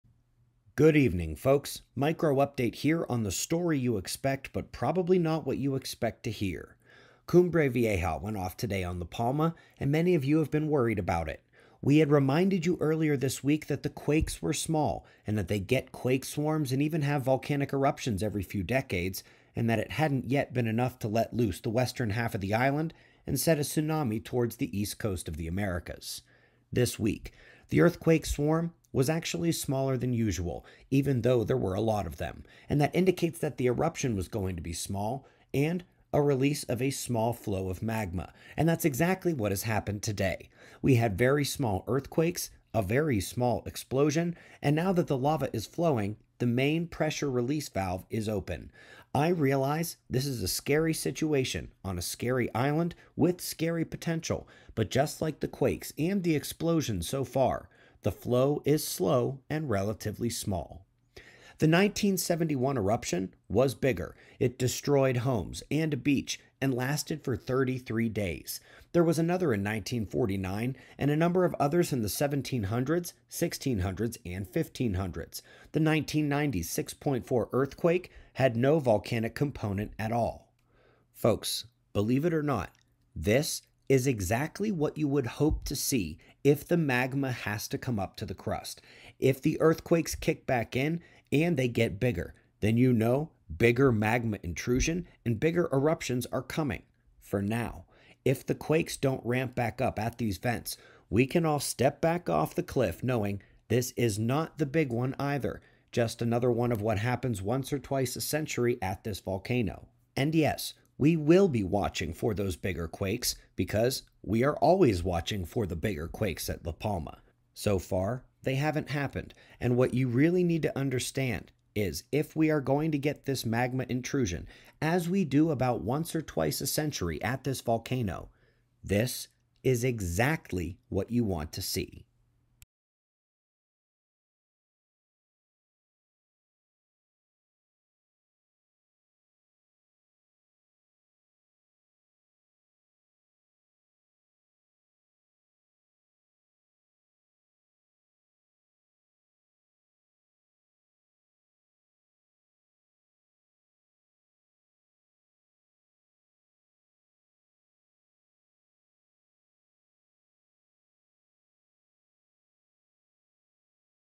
La Palma Volcano | What It Means/Data Update
The video is the nice way of reminding everyone what I said earlier this week.

What I WANTED to say was: Settle the heck down. I told you earlier this week the intrusions were small so far - and if they get bigger we can worry about the tsunami risk. Yes, harmonic tremor swarms approaching the surface means a volcano may erupt... but the size of the quakes tells you a lot.

Yes, we will continue to monitor her, but the quakes are dying down, and until they go up to higher magnitudes the FEAR over this volcano this is overkill.